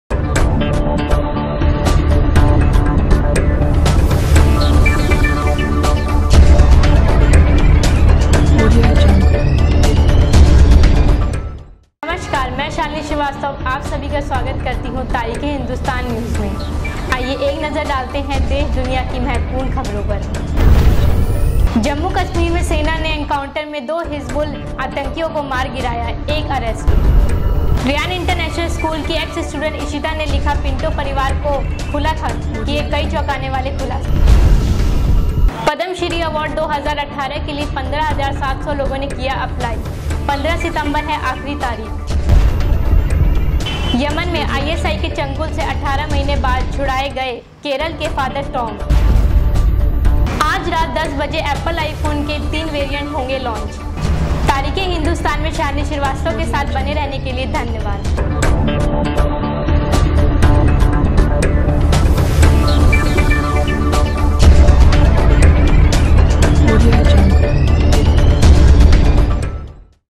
दिनभर की खबरों पर एक नज़र ,तारीख़-ए-हिंदुस्तान न्यूज़ के साथ Shalini Srivastava
Anchor Shalini Srivastava,tarikh-e-hindustan